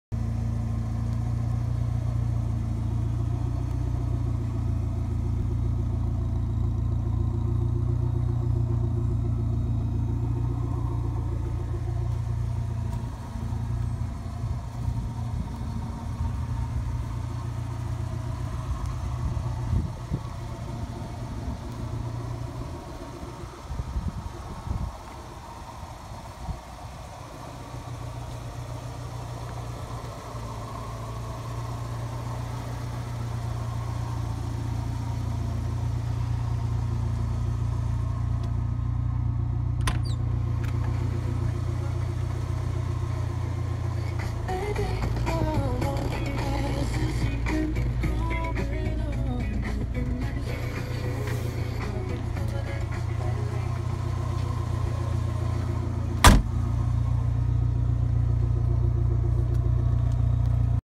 SAAB 900 S Turbo, 1993
SAAB 900 S Turbo, 1993, 218000 km, in Aubergine Pearl colour, interieur grey leather. Airbag, Aircondition, fully service history, last model, runs perfect